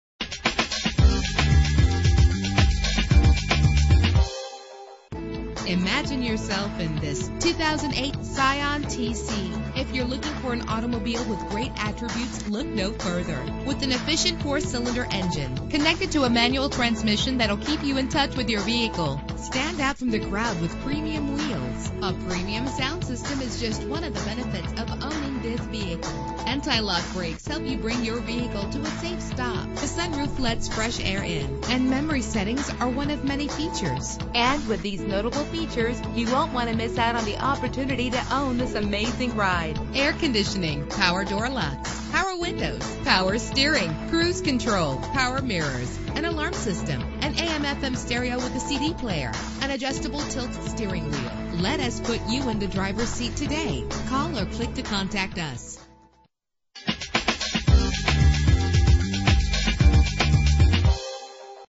2008 Scion tC - Fox Lake IL
Year: 2008
Make: Scion
Model: tC
Trim: Base
Engine: 2.4 liter inline 4 cylinder 16 valve
Transmission: 5-Speed Manual
Color: Waveline Pearl
Mileage: 74535
Address:
75 S Route 12